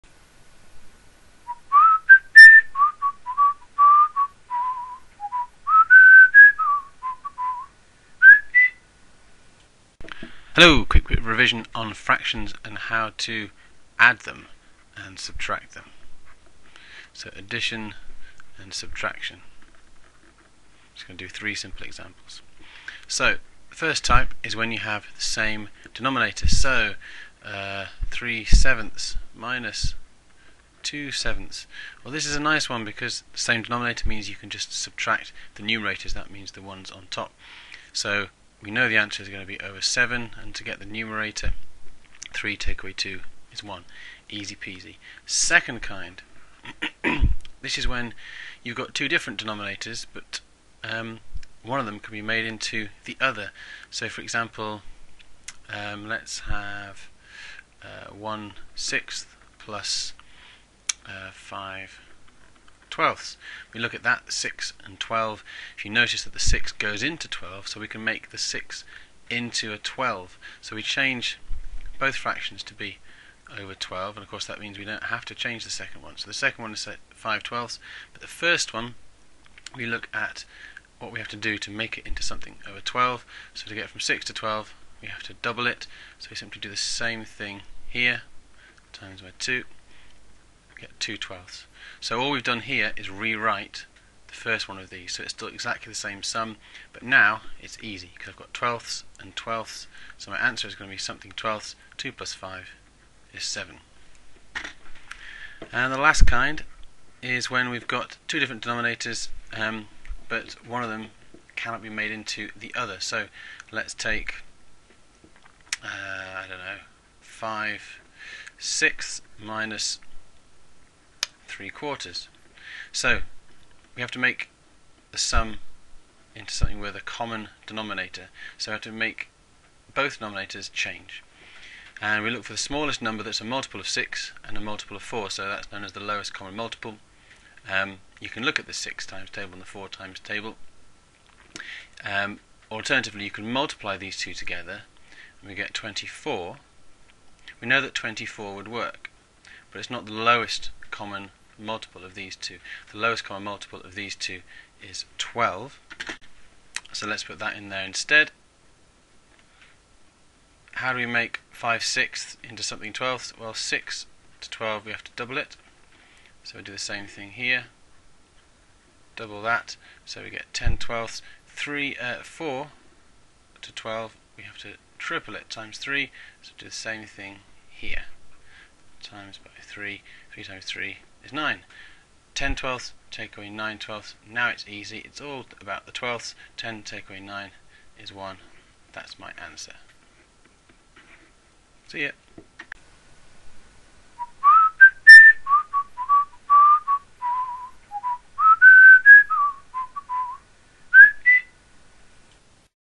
Fractions - Addition and subtraction (quick revision)
What it says on the tin! 3 quick examples of how to add/subtract (proper) fractions.

For the second type, the key is to compare the denominators and see if the smaller is a factor of the greater.

Hope it's useful. Mr C.